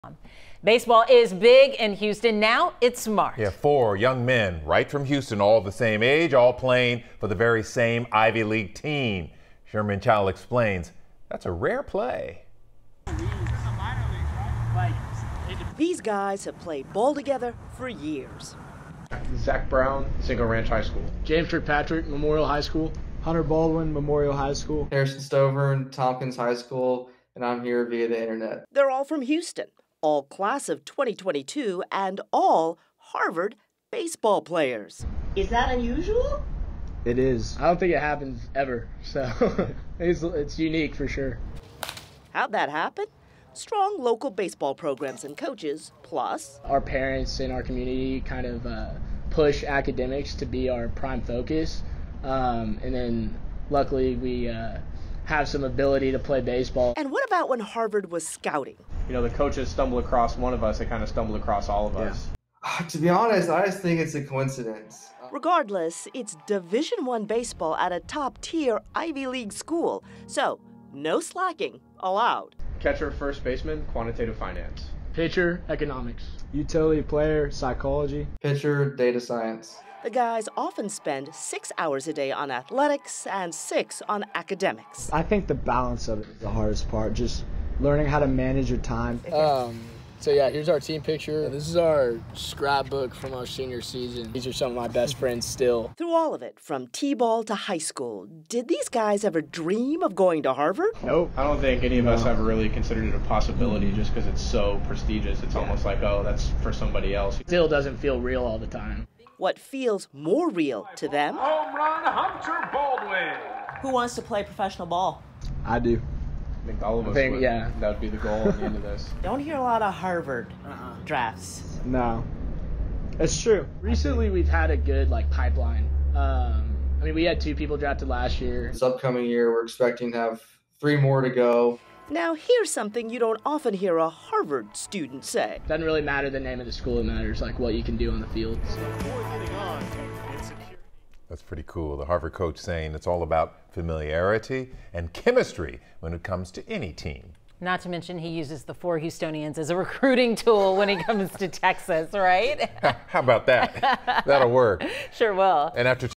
4 Houston athletes headed to Harvard to play baseball
Four teenagers from Houston, who have been playing baseball together for years, are taking their skills to Harvard.